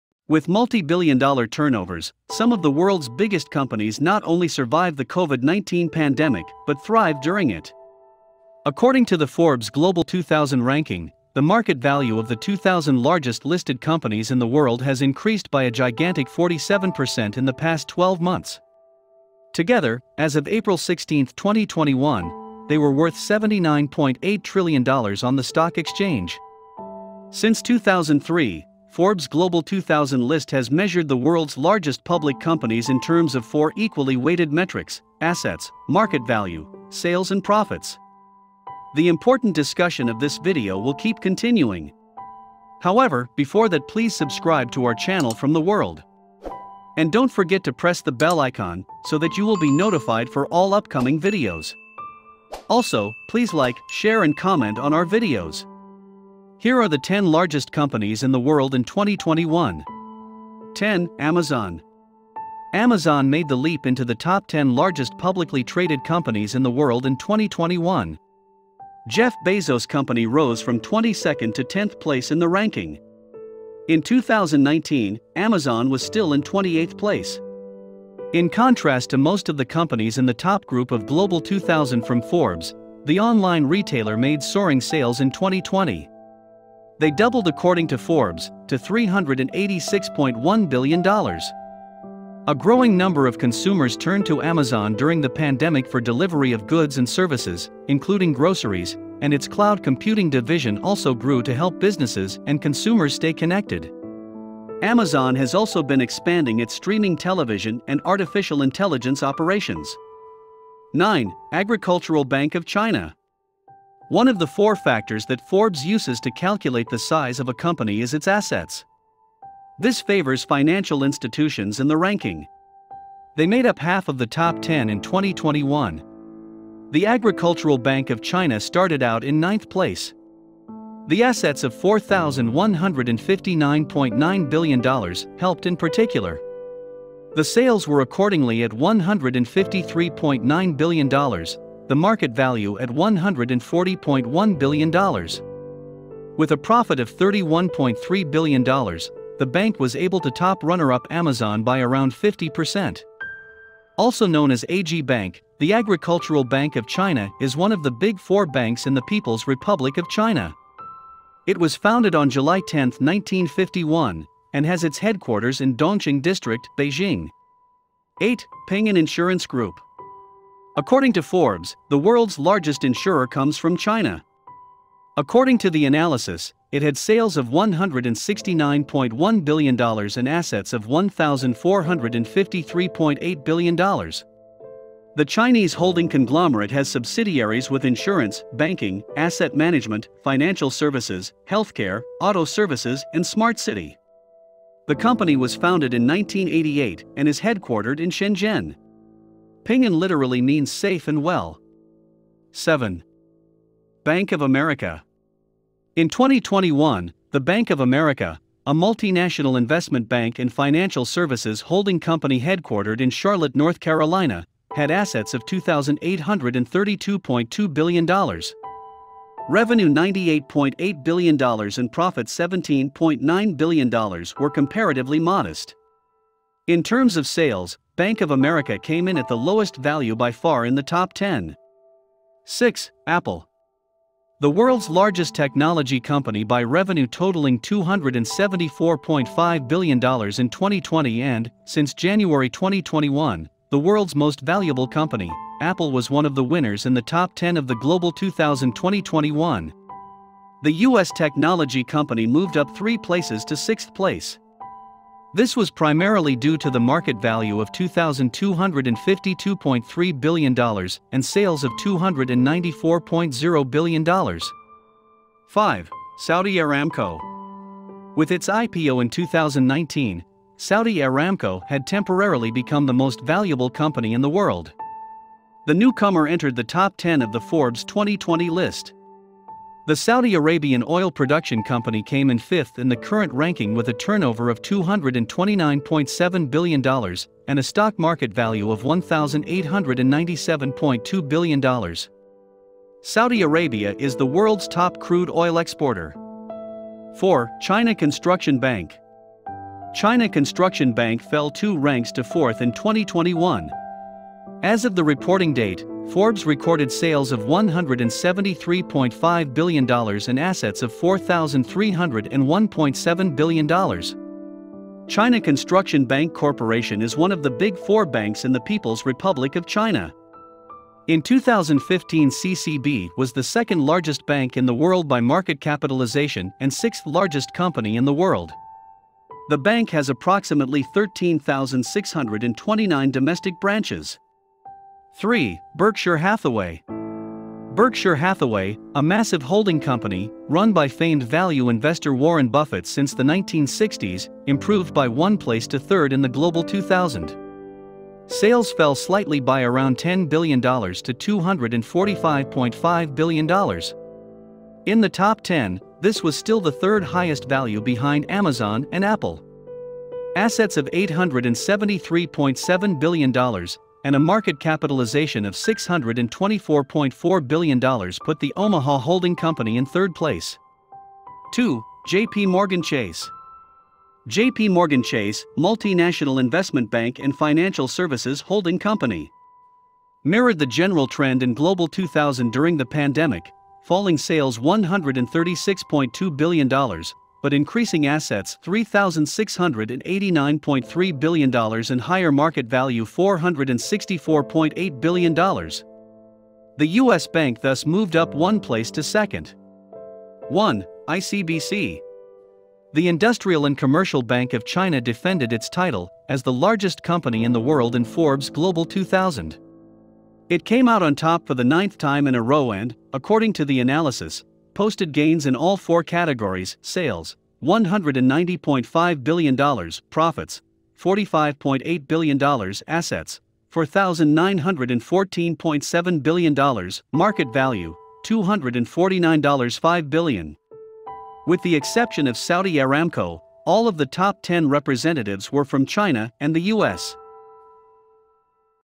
TOP 10 COMPANIES IN THE WORLD HAVING MOST OF THE WEALTH
With multi-billion-dollar turnovers, some of the world’s biggest companies not only survived the COVID-19 pandemic but thrived during it.

According to the Forbes ‘Global 2000’ ranking, the market value of the 2000 largest listed companies in the world has increased by a gigantic 47 percent in the past twelve months. Together, as of April 16, 2021, they were worth $79.8 trillion on the stock exchange.

Since 2003, Forbes’ Global 2000 list has measured the world’s largest public companies in terms of four equally weighted metrics: assets, market value, sales and profits.